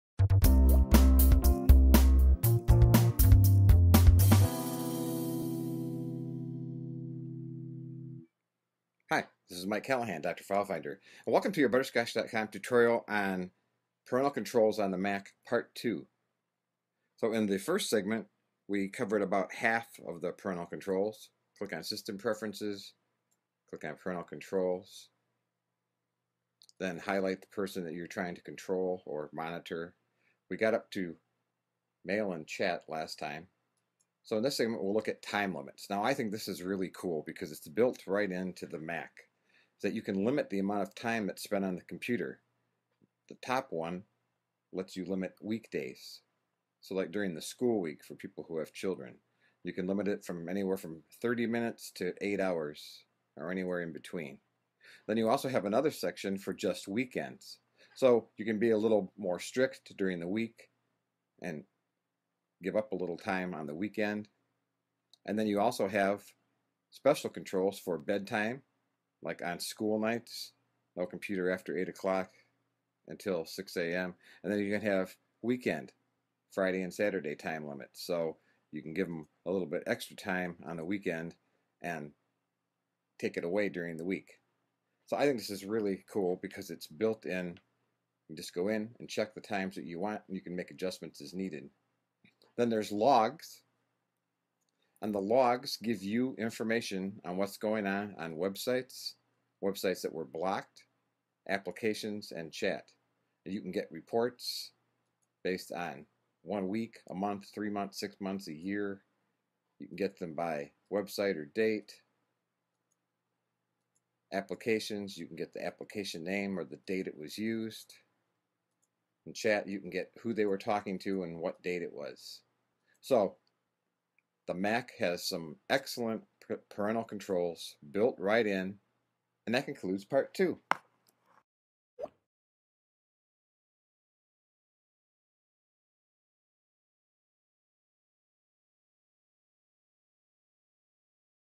How to configure parental controls on your Mac
Parental controls can be a very important tool for parents. The Internet is an amazing place full of knowledge, wonder and communication unlimited. It can also be a dangerous place with hackers, phishers and even predators. Fortunately your Mac has built-in Parental Controls. In part 1 we showed you how to access the Parental Control panel. This is part 2 of 2 of our Parental Controls feature.  A Internet tutorial by butterscotch.com.